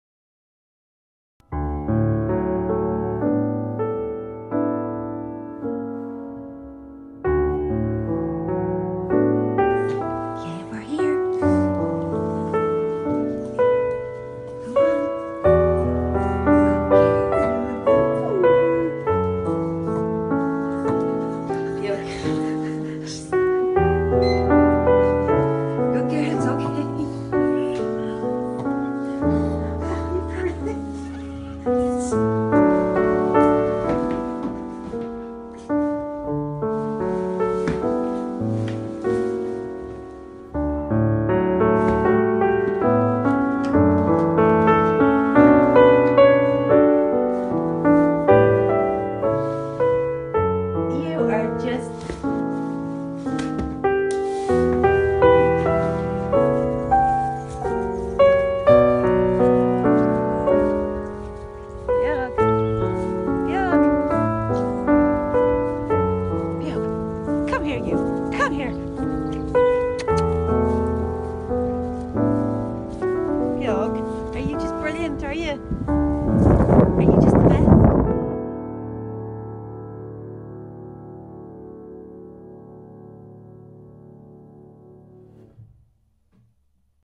Beag the Chihuaha
On the sunny Friday morning of the 2nd of June, Beag the chihuaha seemed quite subdued and out of sorts to her owners Julie and Lynn. However she had went for a ramble around the block with Julie that morning and seemed fine throughout. Later that afternoon their mother had begun to put Beag's harness on for another walk when she let out a painful cry. She was a lot slower and more cautious on this walk. When Lynn came home that evening and went to pick Beag up she screeched out in pain again. She was then rushed to the vet straight away. By the time they arrived Beag had already lost the power in her back legs and could no longer stand up.

Beag was referred straight to Dr.McCartney in NOAH with a suspected spinal injury. Dr.McCartney carried out an MRI scan on Beag and she was diagnosed with a lumbar disc extrusion; a serious spinal injury requiring an emergency surgery.

After a very successful surgery Beag then began rehabilitation at our sister clinic Malahide Veterinary Clinic, where she underwent physio and hydrotherapy using our in-house under water treadmill.
We were delighted to receive an update from Beag's wonderful owners, check out the video below to see Beag's progress!